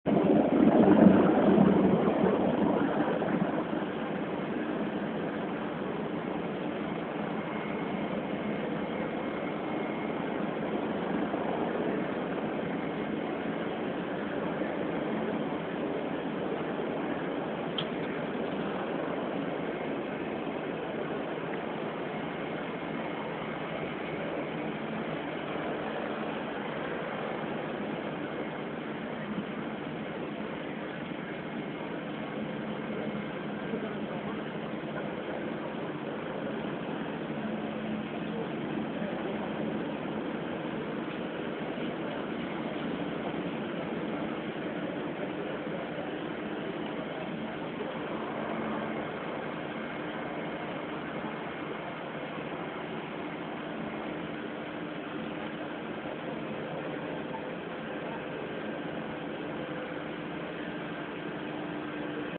ALDWYCH FIRE LONDON - V17 HP 6
ALDWYCH FIRE LONDON 14TH JUNE 2011 - MOBILE FOOTAGE